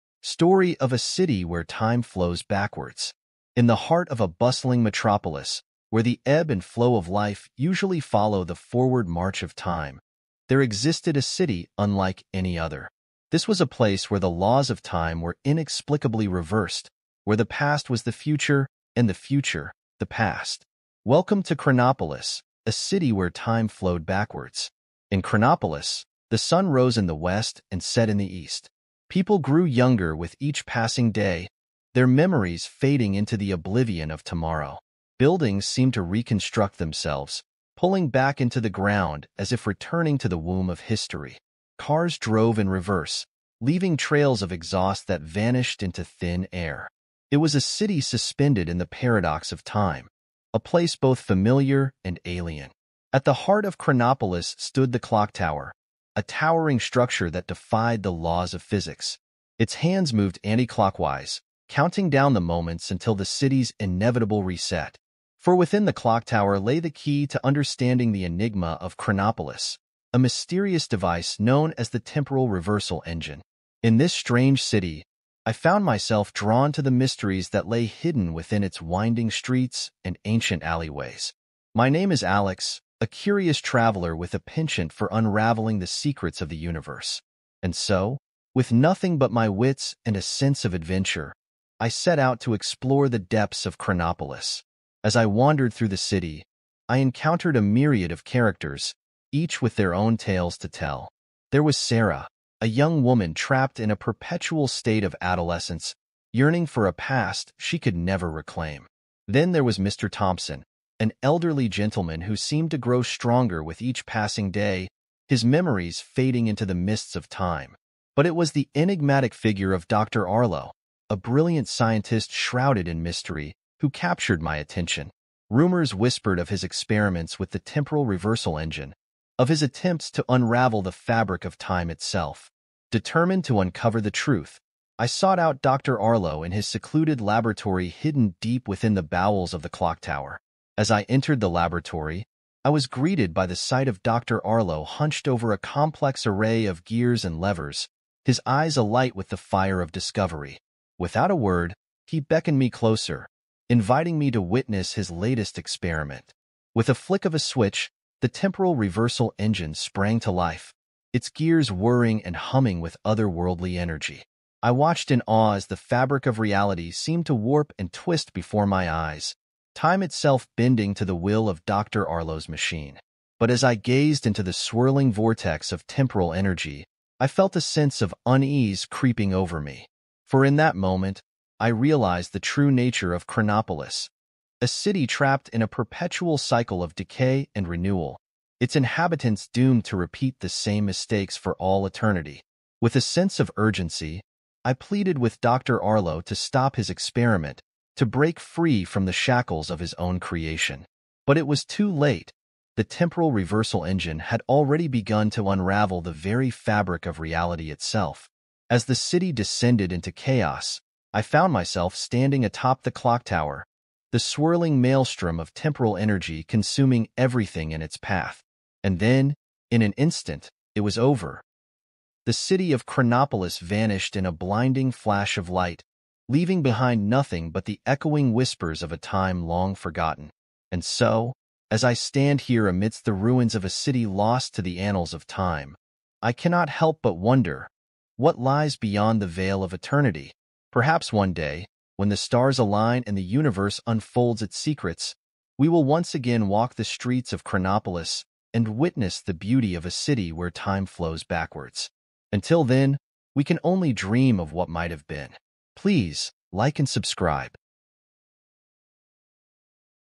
Mysterious City: Time Runs Backwards Here
Step into the mysterious city where time runs backwards. Watch as the clock hands turn in reverse and discover the strange secrets of this unique place where time flows backwards. Are you ready to experience the magic and mystery of this mysterious city?